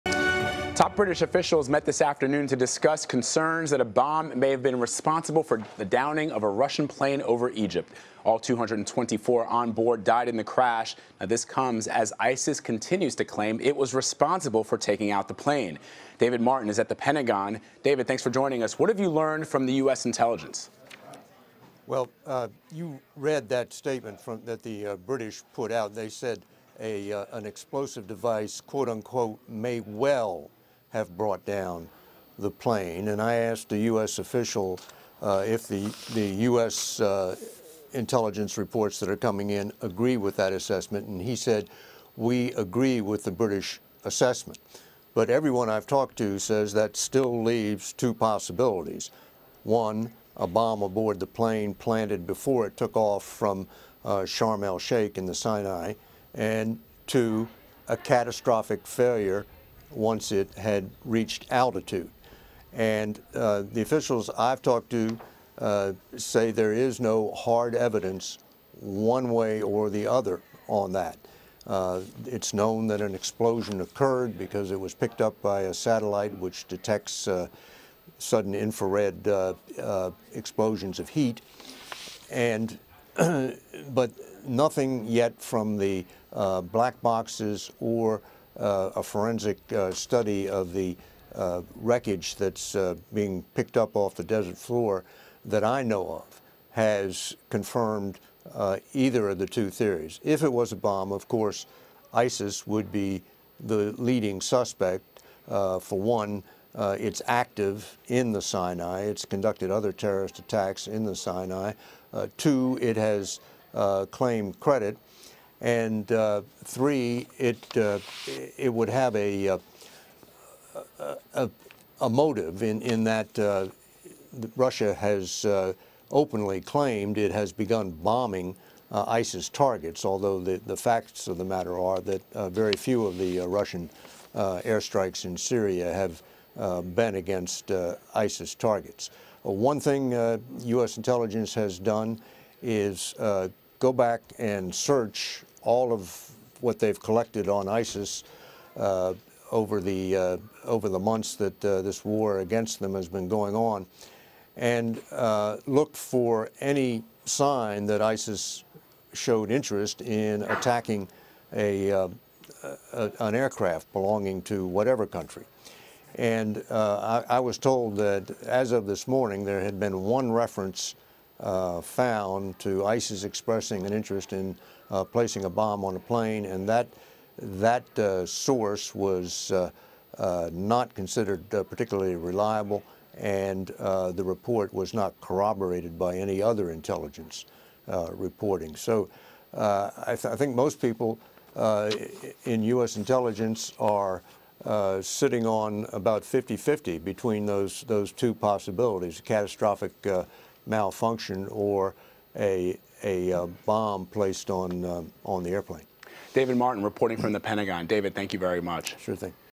US Intel: ISIS bomb possible in downing of Russian plane
U.S. intel says there's a possibility that an ISIS bomb may have downed a Russian plane over Egypt that killed all 224 passengers. CBS News national security correspondent David Martin joins CBSN with the latest.